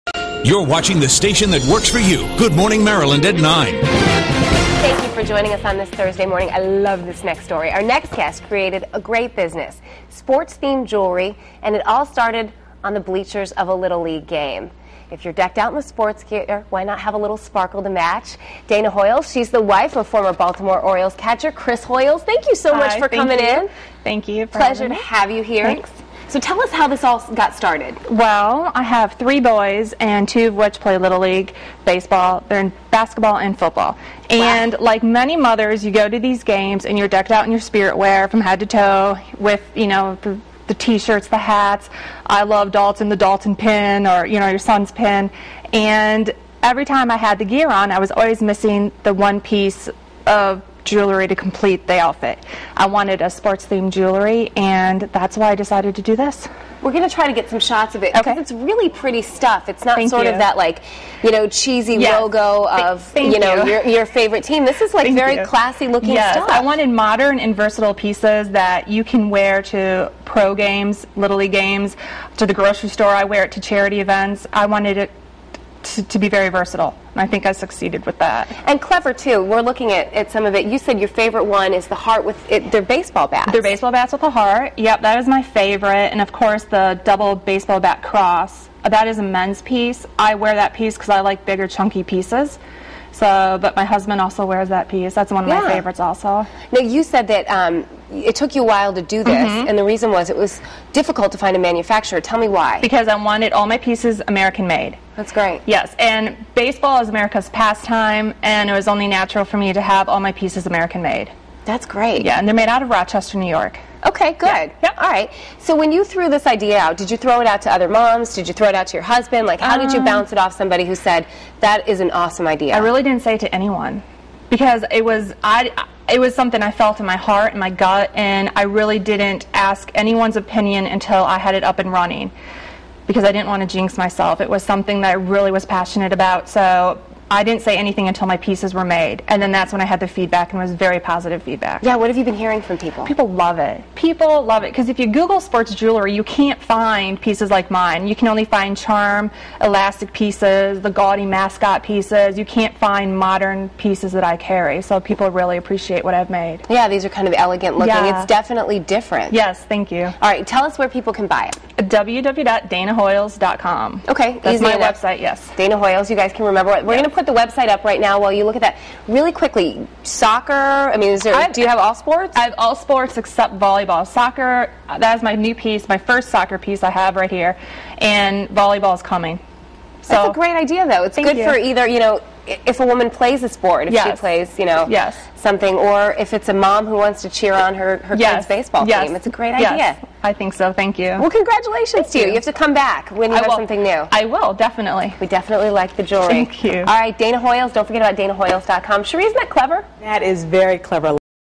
Dana Hoiles Sports-Themed Jewelry
Dana Hoiles, wife of former Baltimore Orioles catcher Chris Hoiles, talks about the inspiration behind her decision to create sports-themed jewelry for other moms.